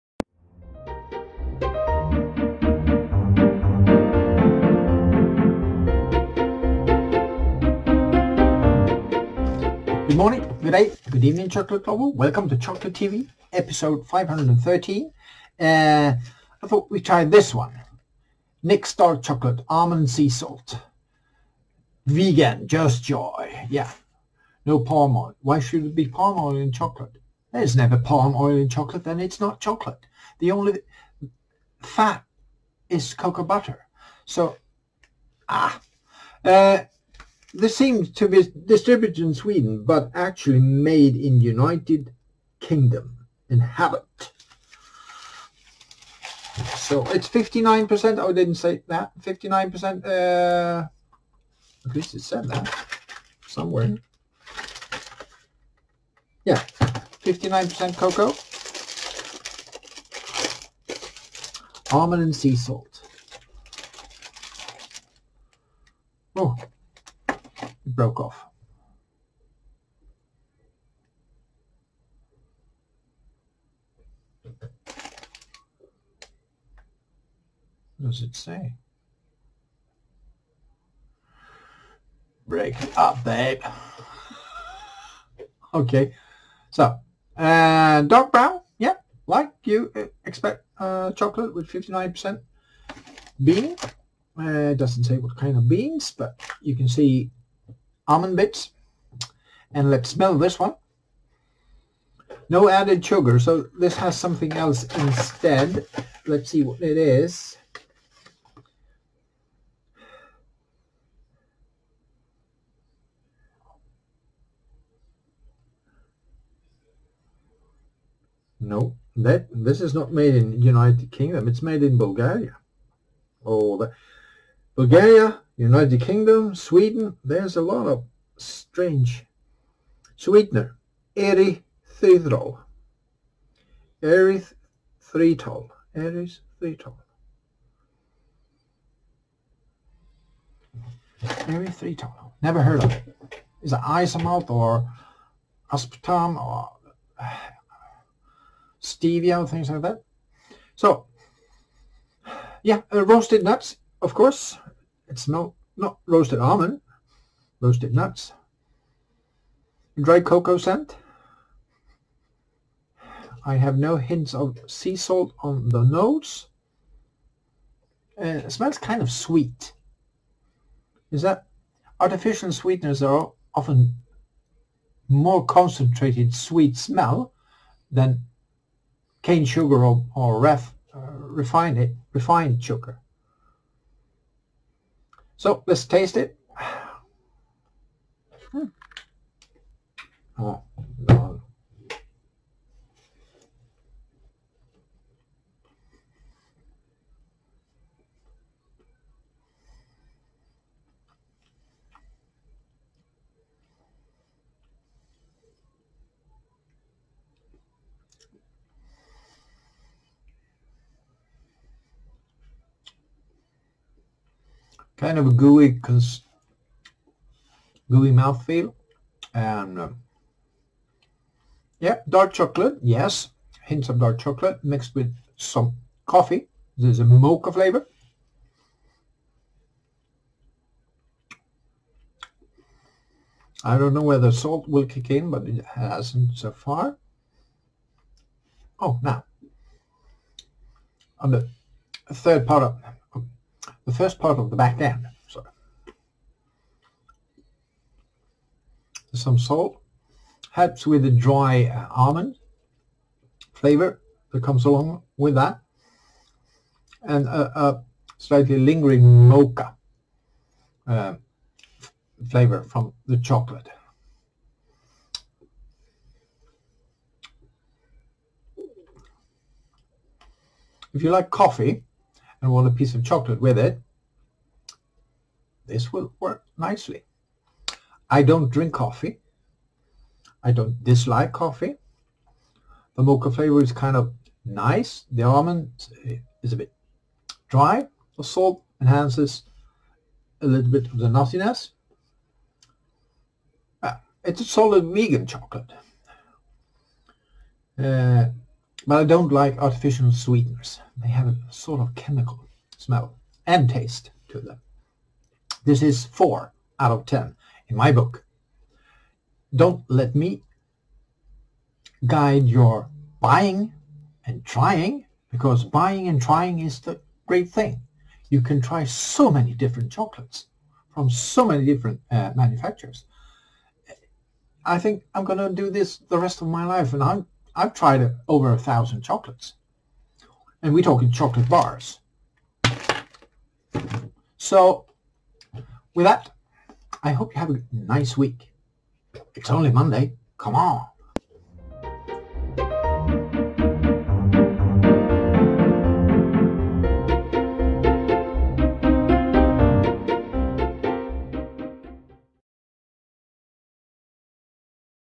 ChocolateTv episode 513
Vegan safe but with artificial sweetner. Makes the chemical aspect a little bit to evident despite the chocolate have a lovely mocca flavour.
Made in Bulgaria, by an UK firm for a Swedish company??? It is a global market, chocolate.